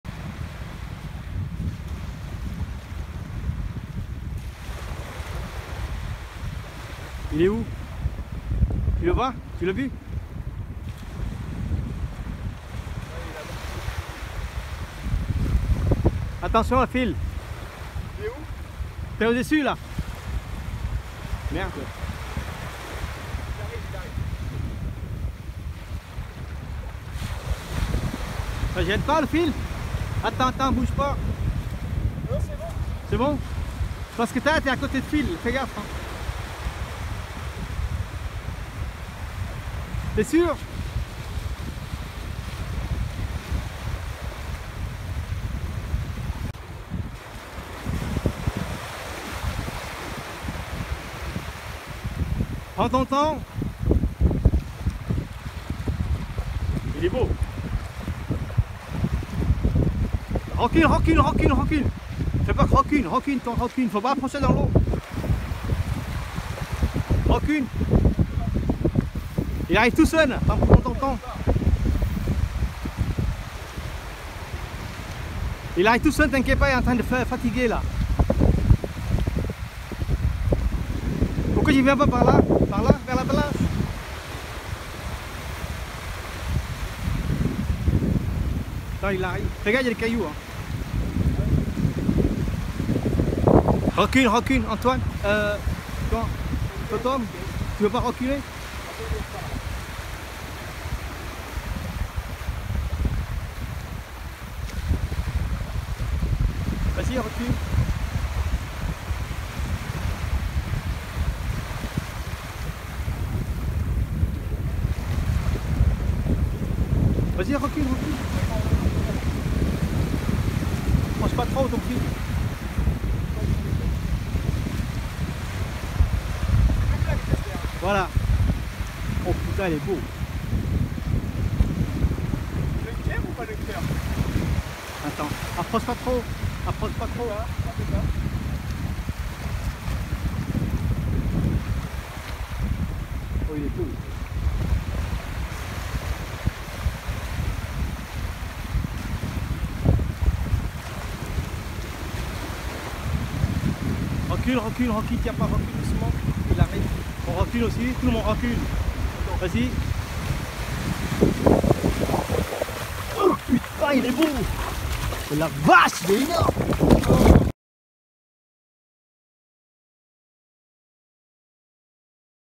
Bar de 85 en surfcasting dans la Manche
Après le confinement, nous sommes à la recherche des beaux poissons. Nous profitons des grandes marées de juin sur un spot que l'on connait un peu. Une météo maussade mais pas trop de pluie ni d'algues. De nombreux appâts d'essayés mais nous aurons une seule touche. Touche sur un beau casseron de 100gr.